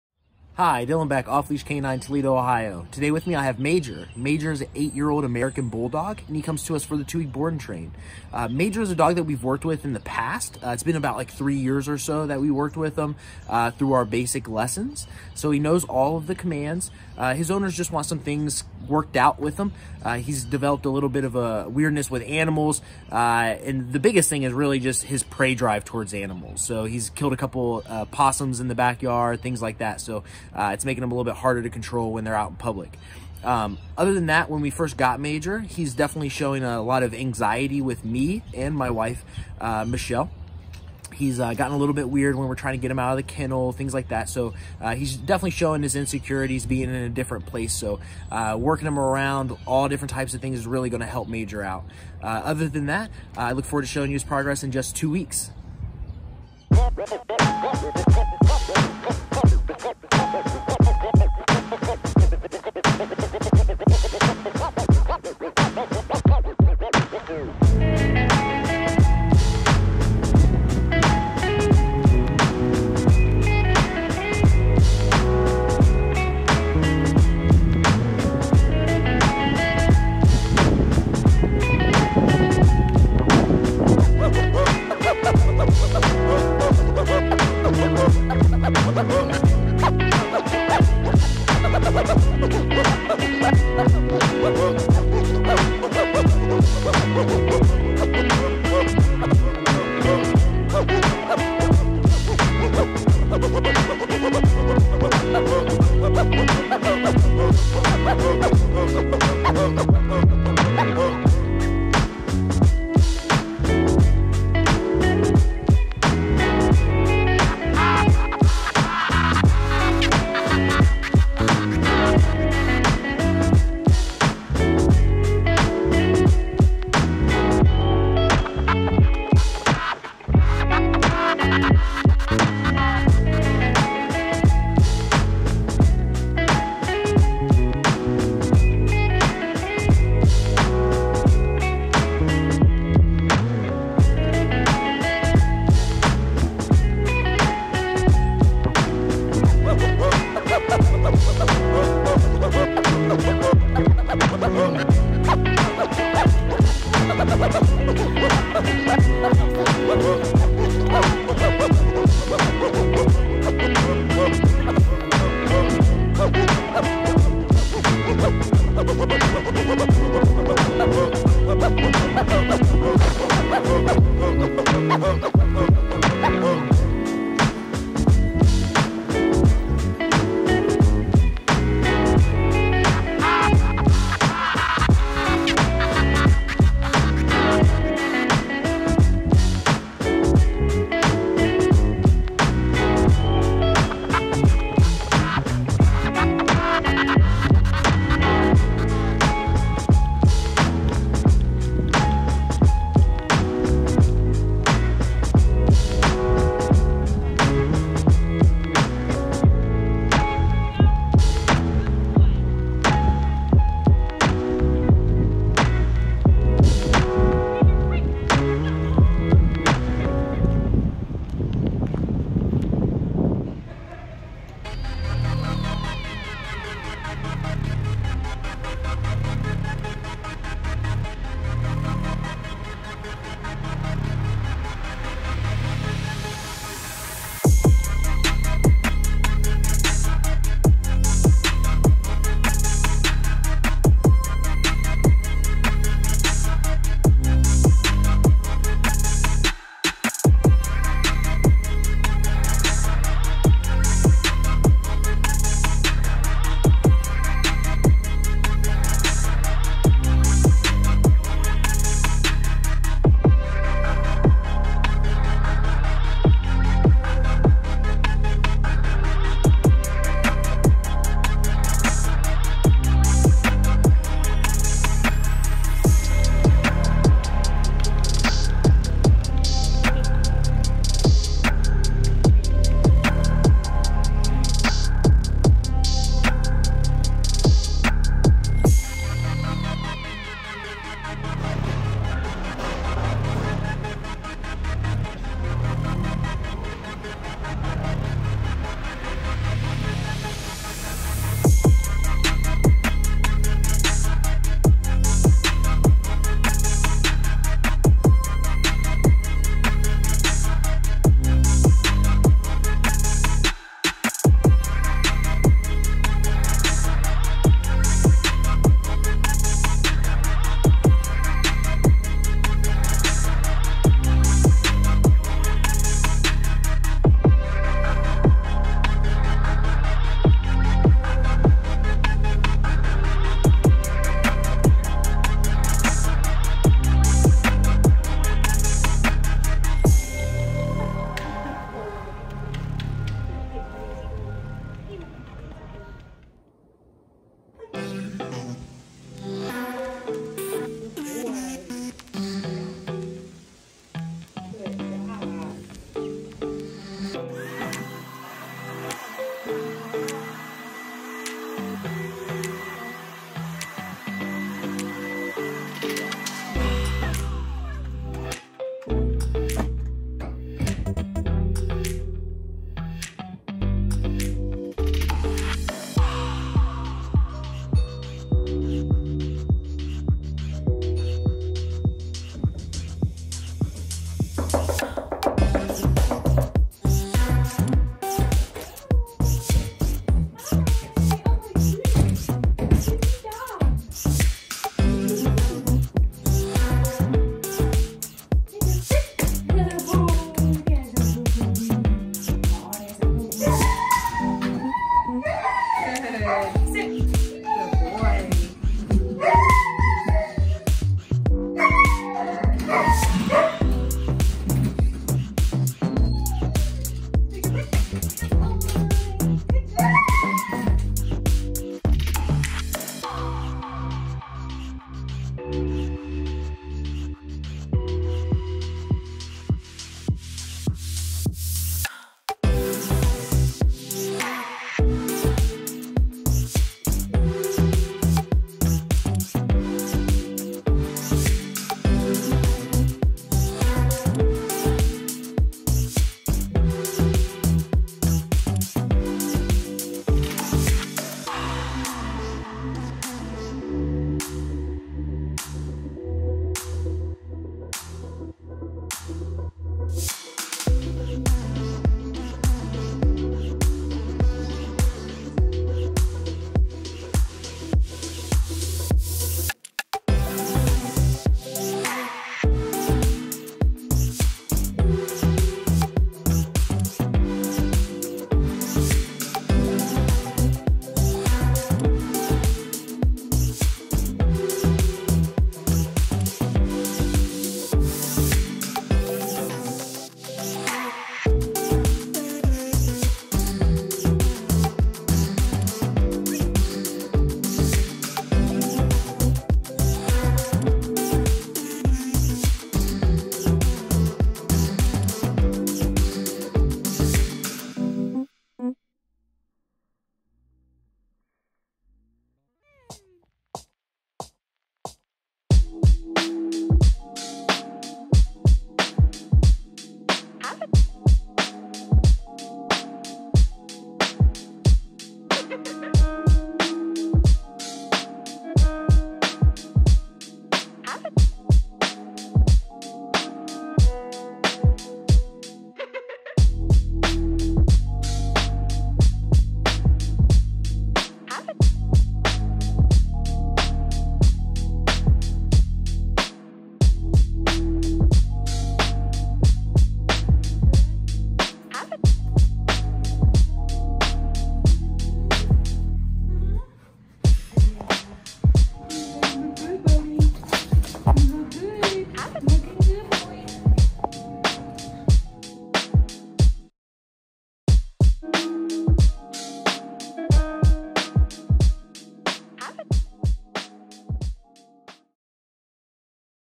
Best Dog Training Toledo, Ohio! 8 Year Old American Bulldog, Major!
Take a look at the progress we were able to make with Major in just 2 weeks!